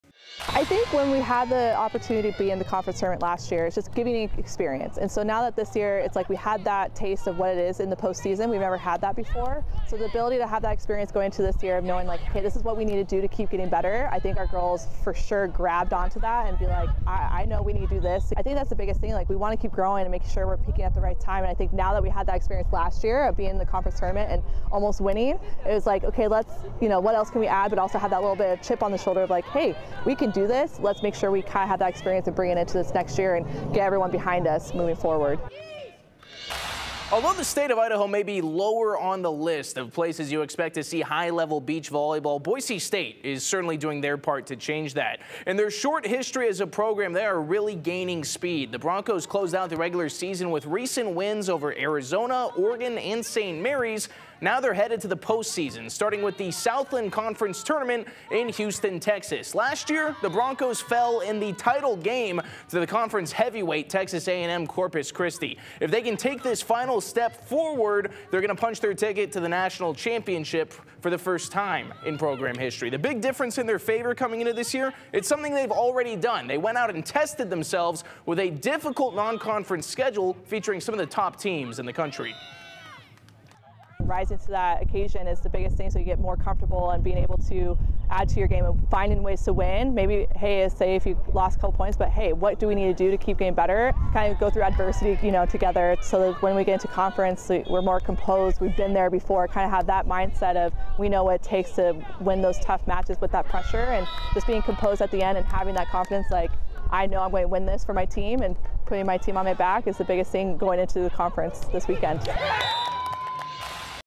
Boise State beach volleyball prepares for Southland Conference Championship
Boise State finished the regular season 28-11 overall and 6-1 in conference play. The No. 2 seed Broncos face No. 7 McNeese State in the first round Friday.